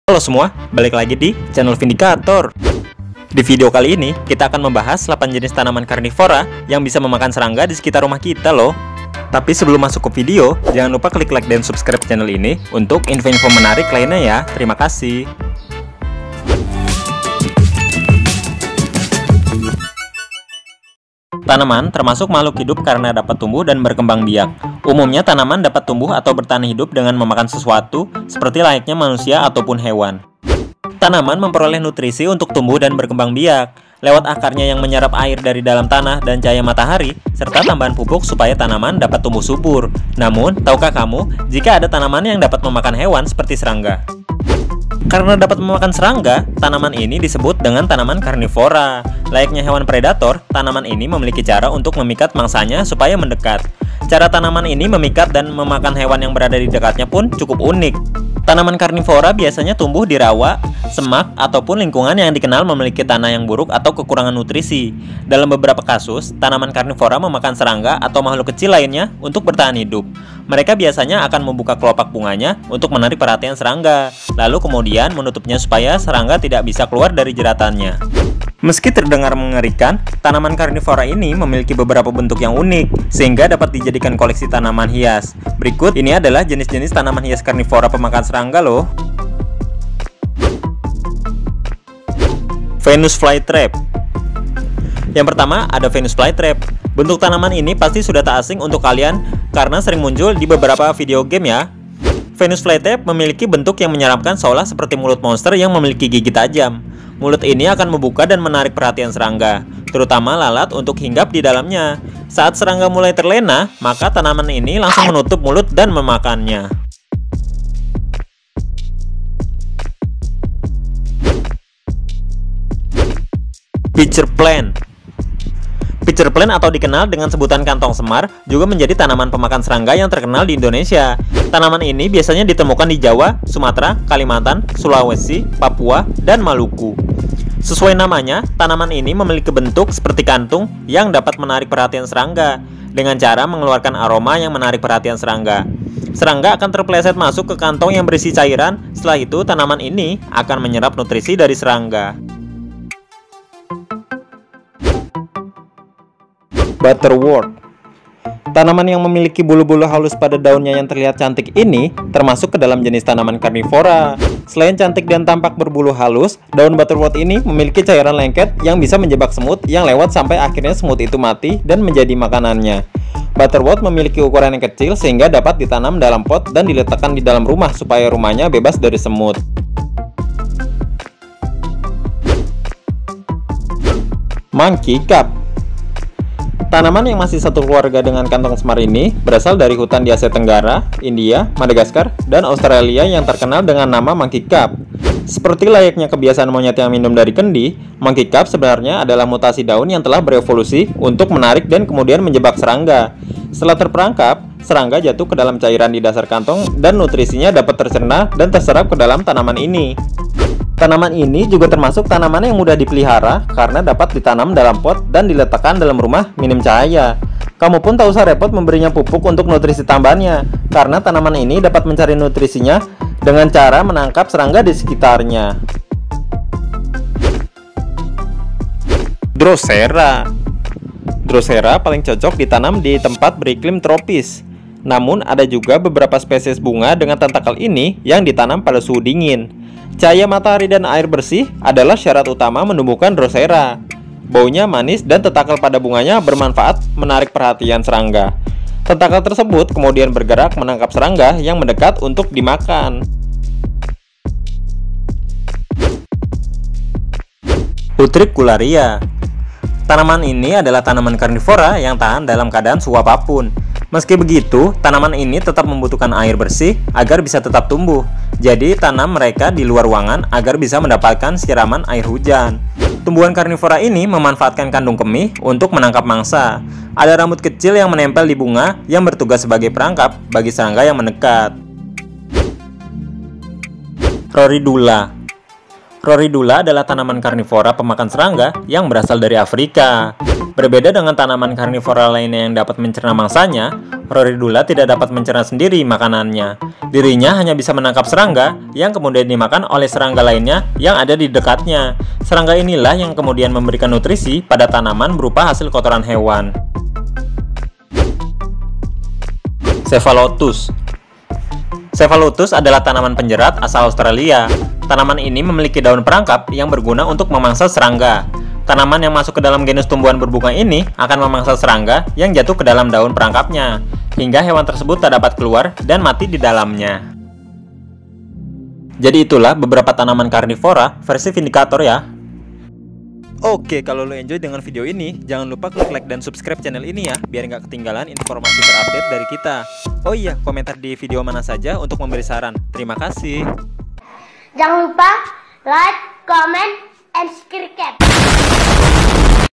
8 TANAMAN KARNIVORA PEMAKAN DAGING YG MEMPUNYAI JEBAKAN BERBEDA BEDA
*Komentar di video mana saja untuk memberi saran

Pastinya TERIMAKASIH Sudah Klik video ini, Semoga banyak rezeki nya dan dilancarkan usahanya, Sampai Jumpa